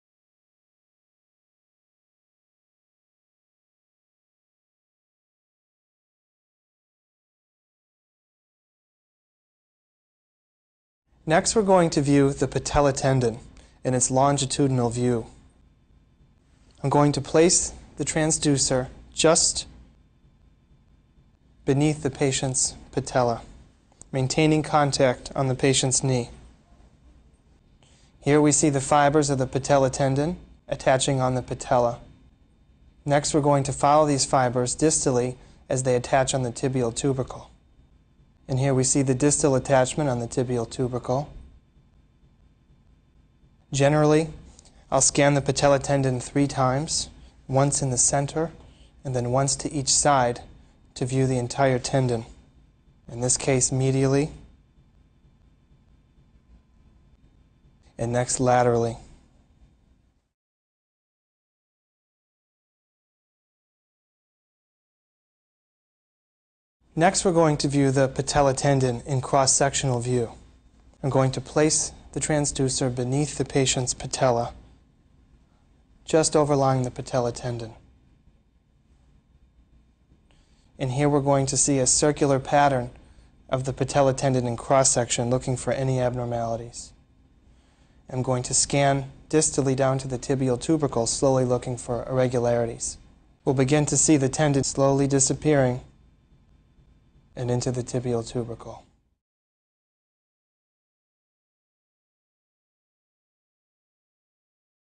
Comment : Réaliser un examen échographique du tendon rotulien
Examen du tendon rotulien comprenant une discussion sur la position du patient, la position de la sonde, les paramètres de l'échographe et la technique d'acquisition.